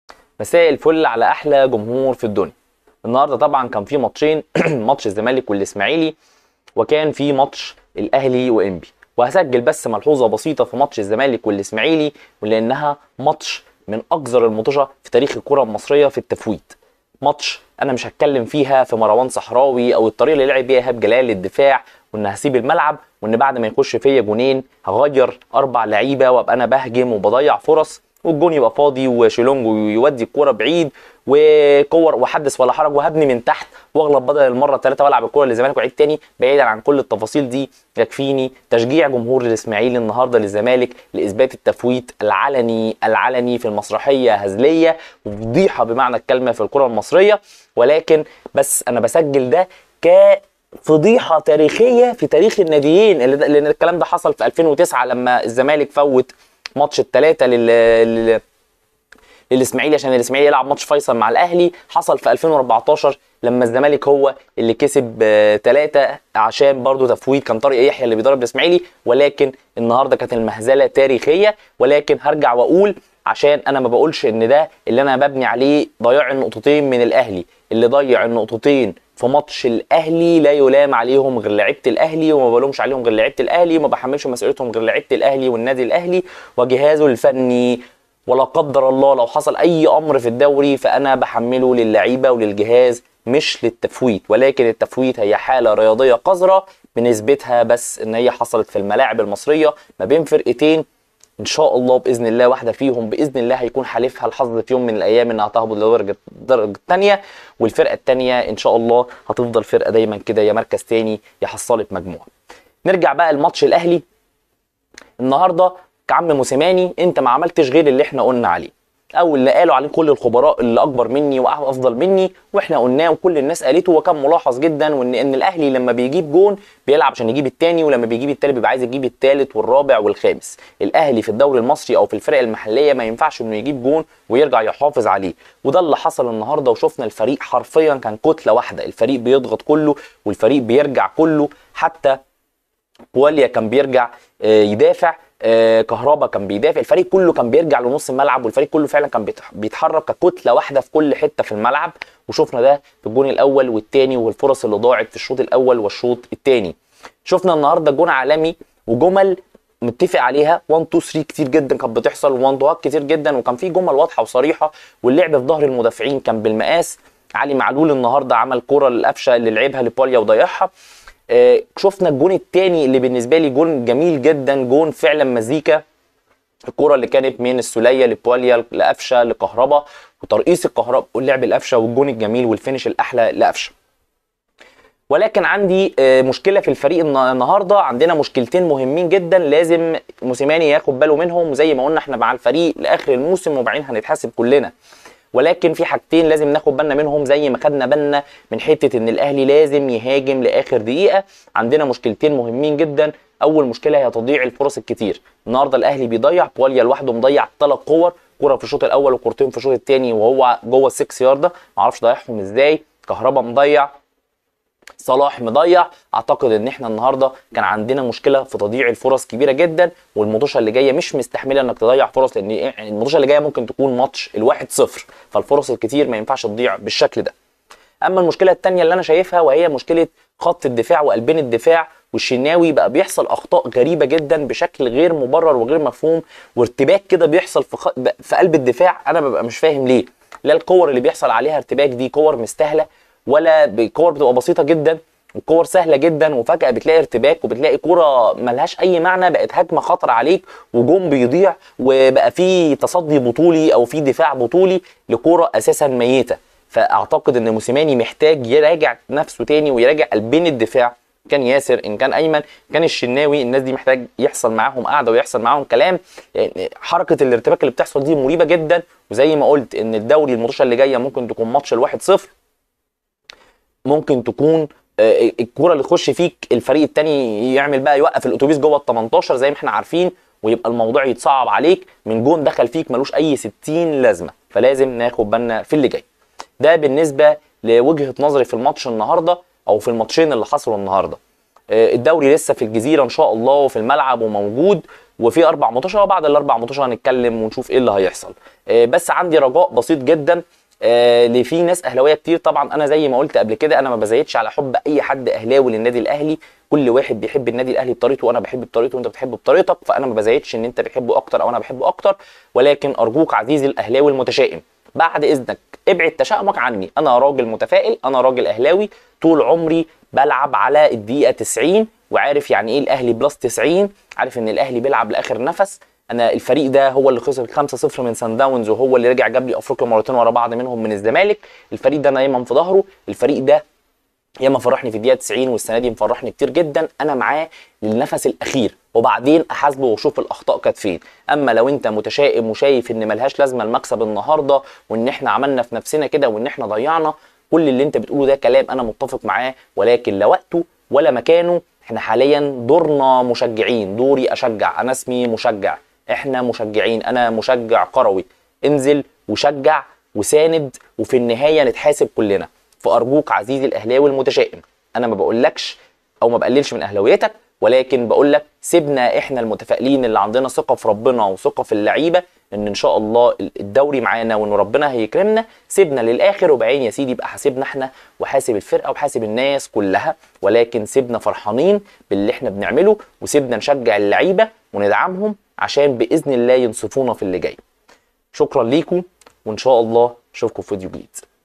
الاهلي و انبي فوز مهم | وفضيحه في الكوره المصريه ( التفويت )
فوز النادي الاهلي علي انبي وتفويت الاسماعيلي للزمالك في فضيحه كروية